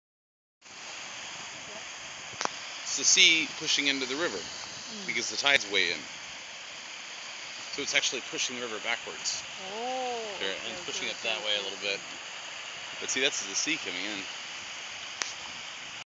A strong tidal surge pushes the Daet River backwards!
Seen from the Mercedes side of the yet to be completed bridge linking the community with Bagasbas Beach,  the high tide and tidal surge pushes the Daet River backwards.